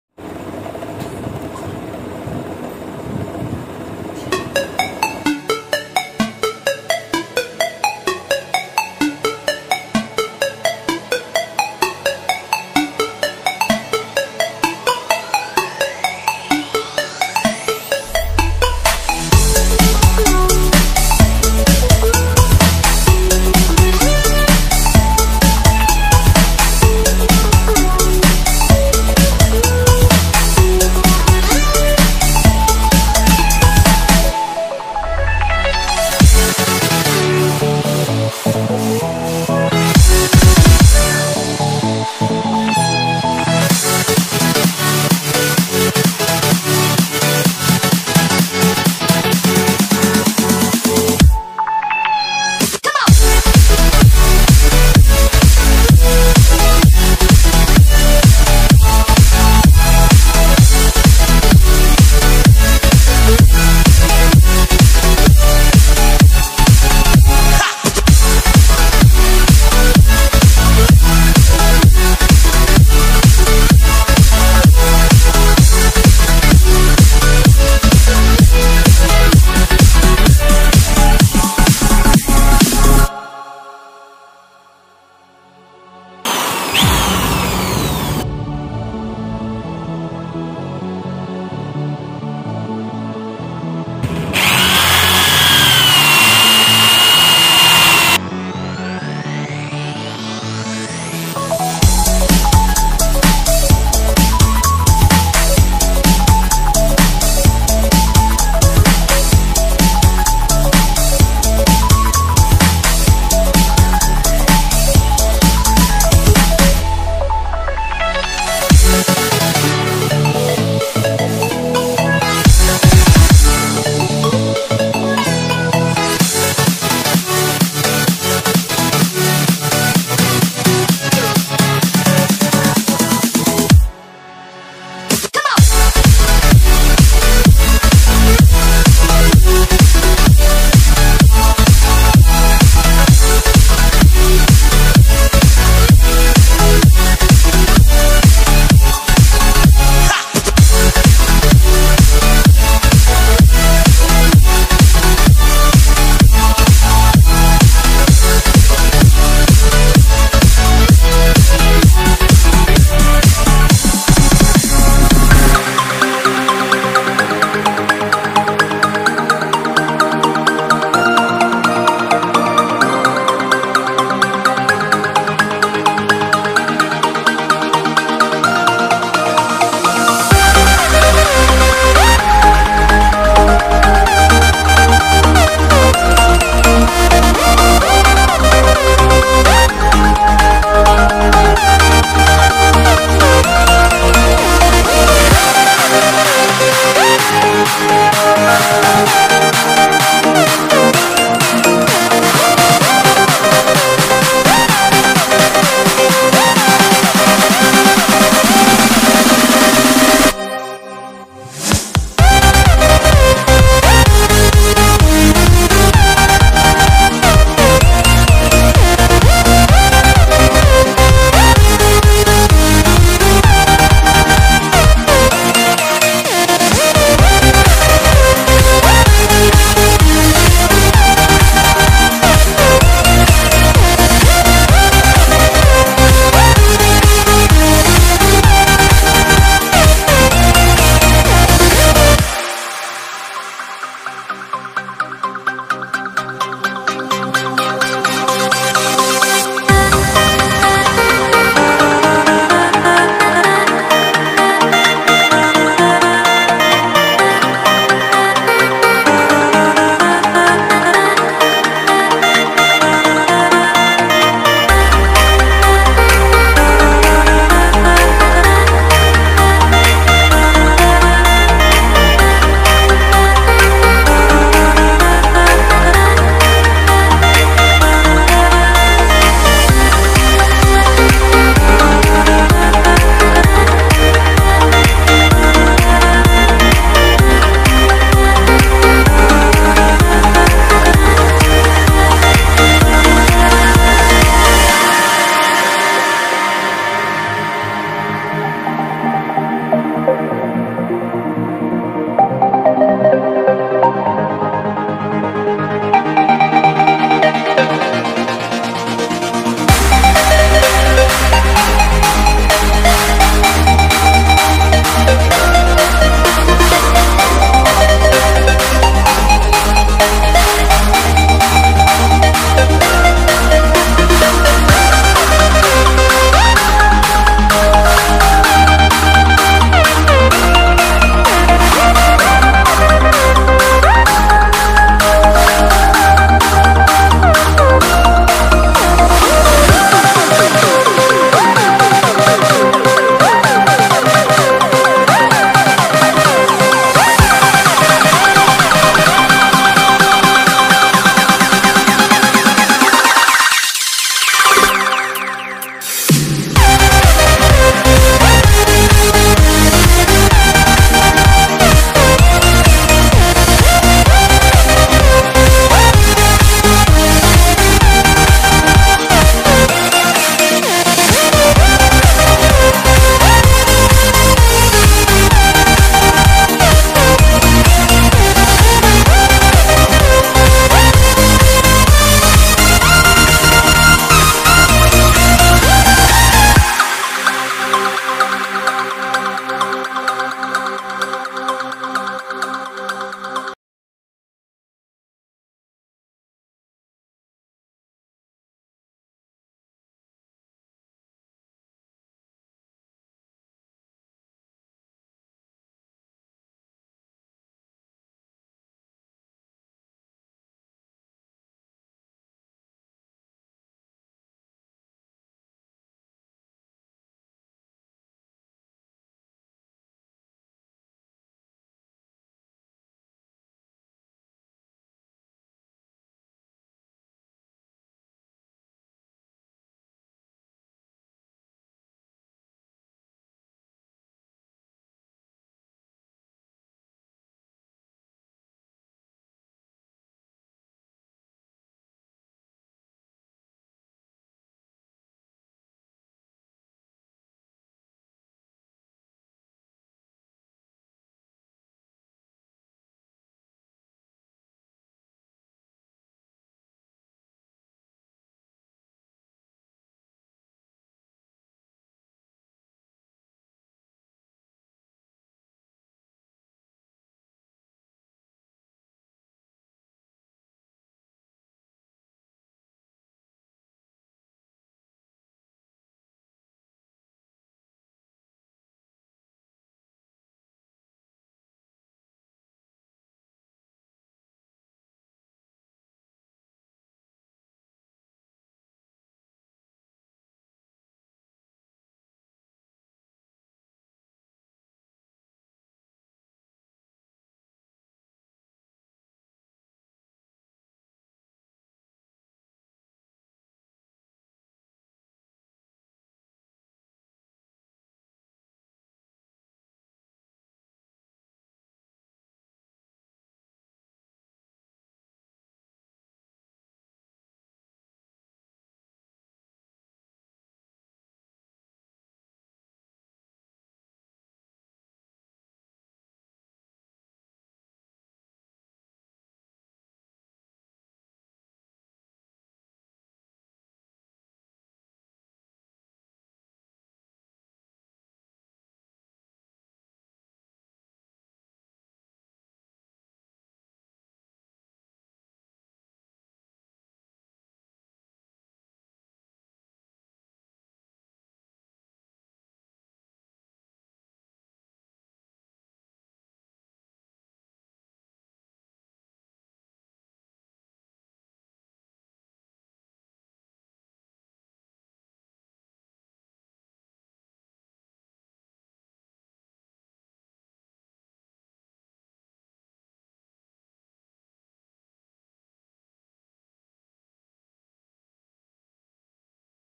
How to turn copper coil into a high power 230v electric generator
How to turn copper coil into a high power 230v electric generator - amir experience - free electricity new 2021

Hello guys in this video I'm showing you about free energy generator

KEEP Supporting 😊💓✌️✌️💕💕💕💕